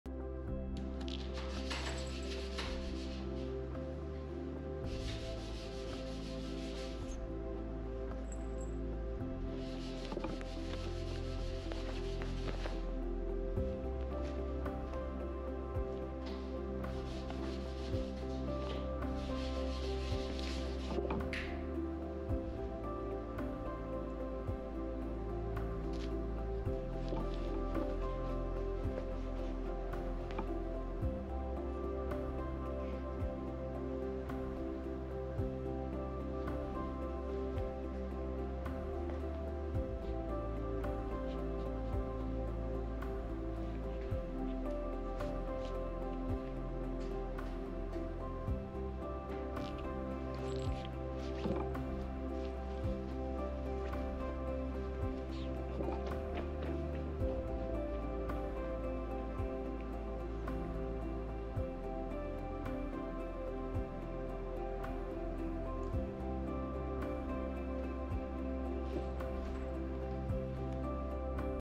Cute cat video!! Calliope and her blankie!
Callie got a chewy goody box and she is loving everything about it!